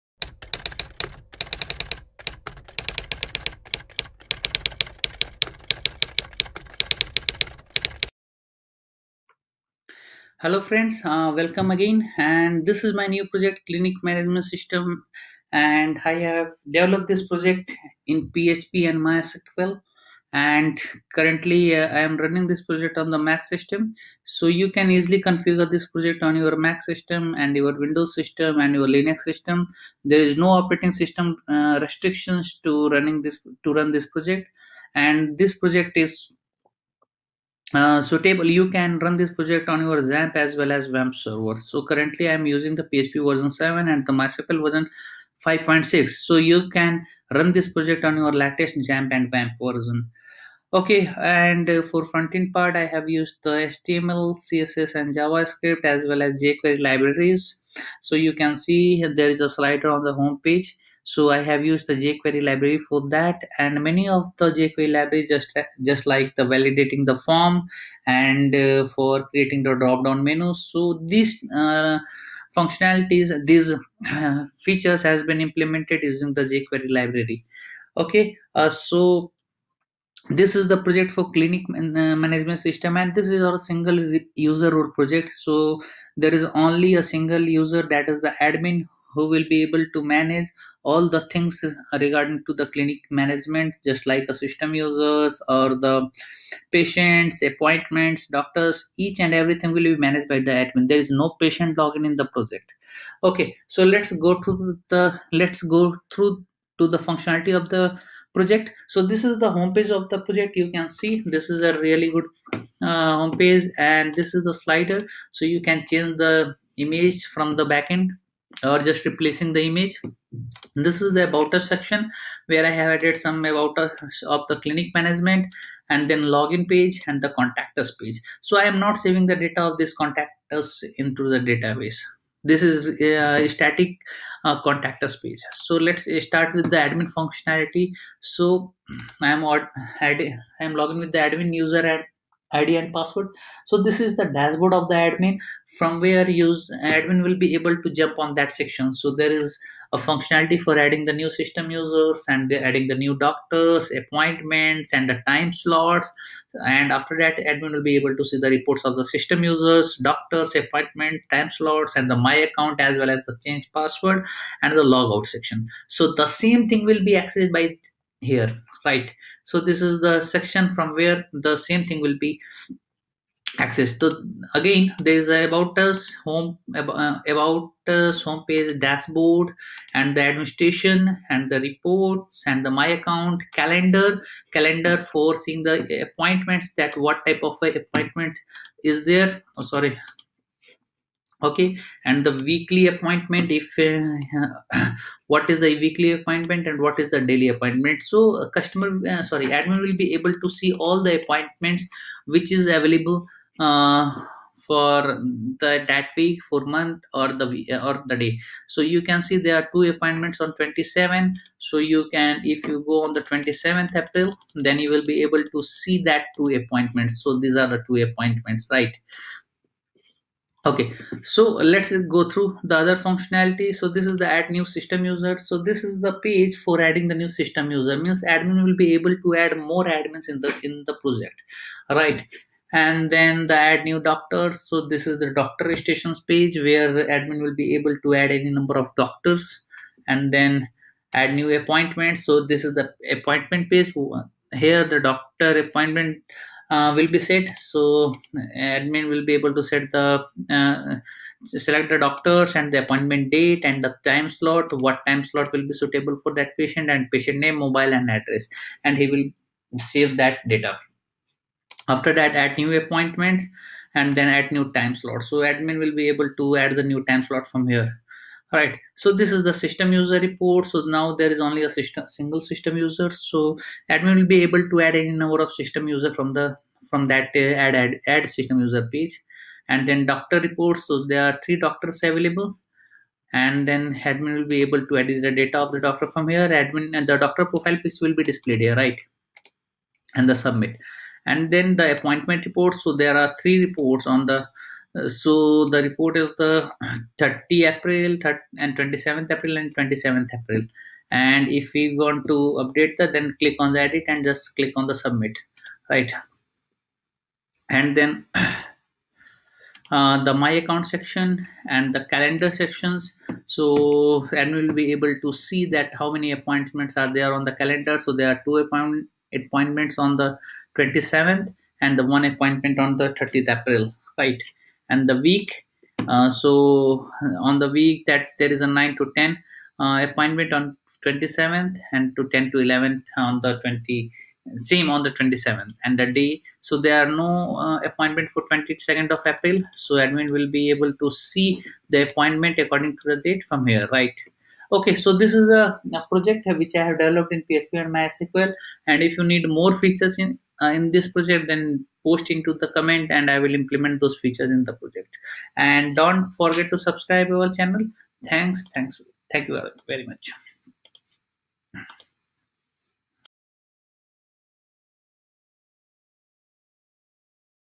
Clinic Management System | PHP and MySQL Project Source Code | PHP MySQL CRUD Project
Clinic Management System is a PHP MySQL micro service project where all APIs we have developed in PHP MySQL and all UI Clinic Management System  we have implemented in PHP.

Clinic Management System PHP and MySQL Project Tutorial
Clinic Management System Download